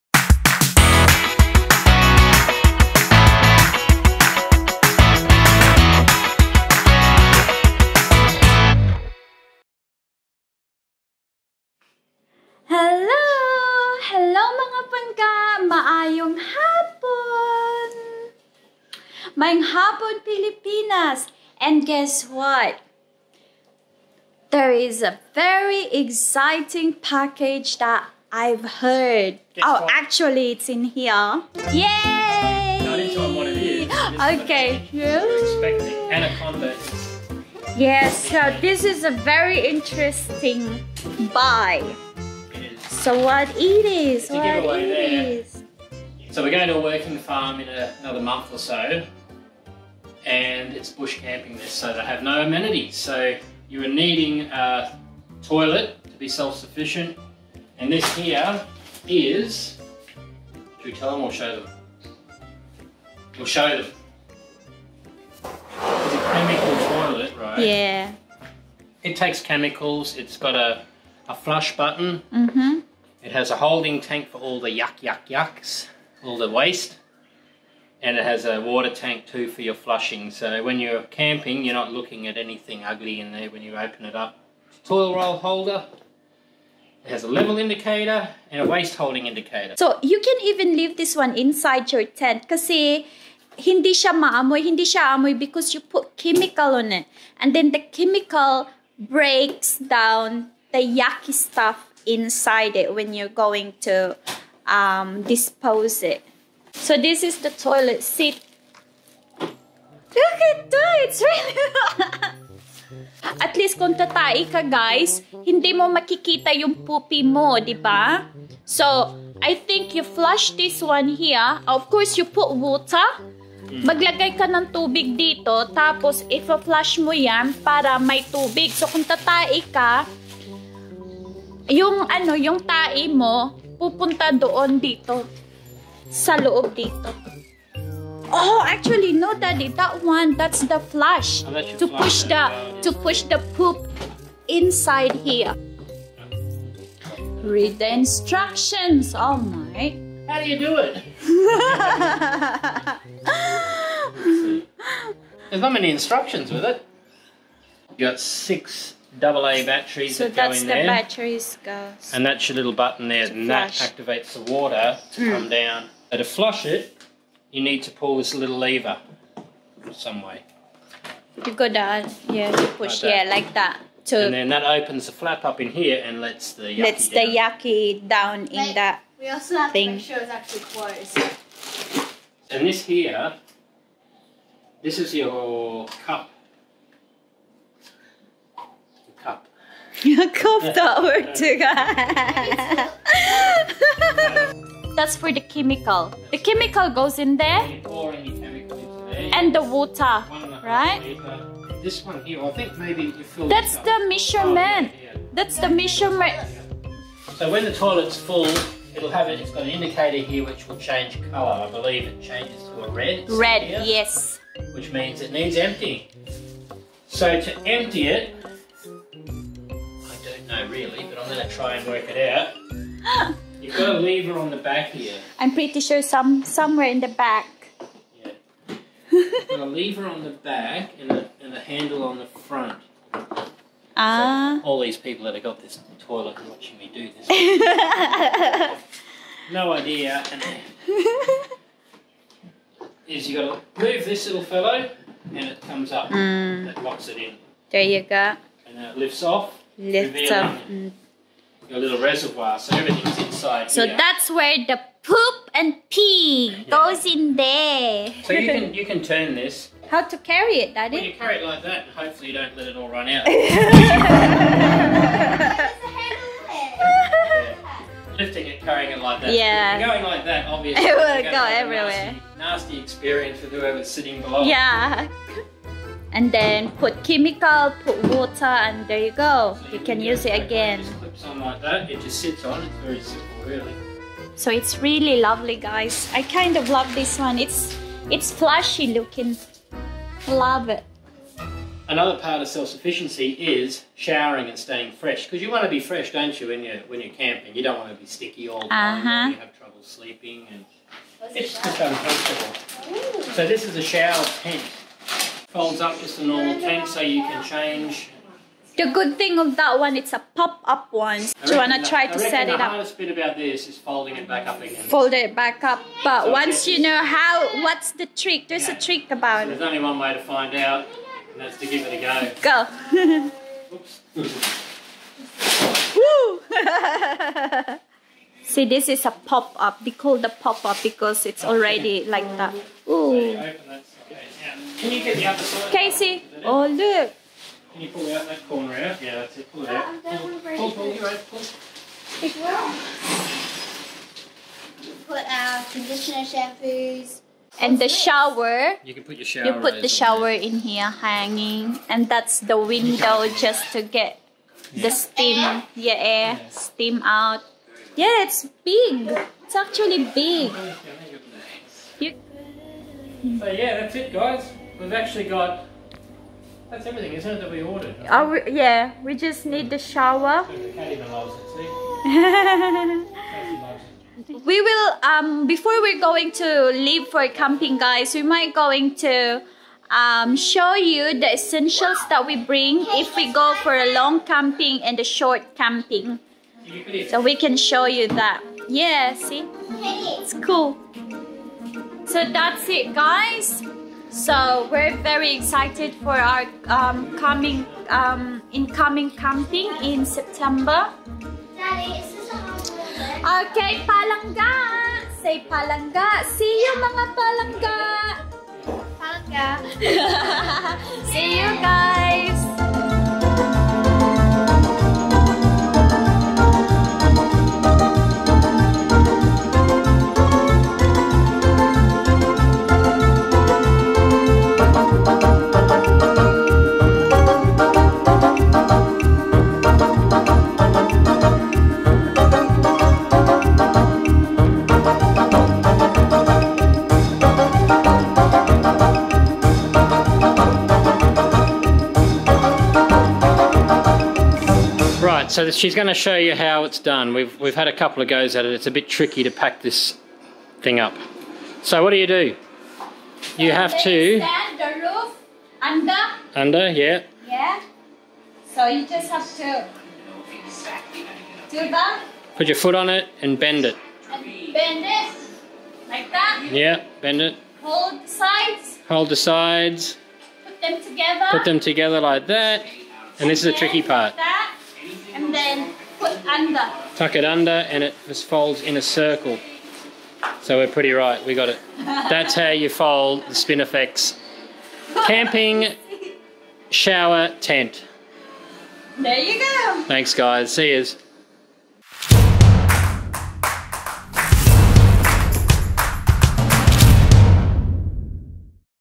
The Stones Life - Outback Essentials For Bush Camping
A last minute package has arrived for us just before we start our week camping in the North Queensland Outback at Hurricane Station. I wonder what it is???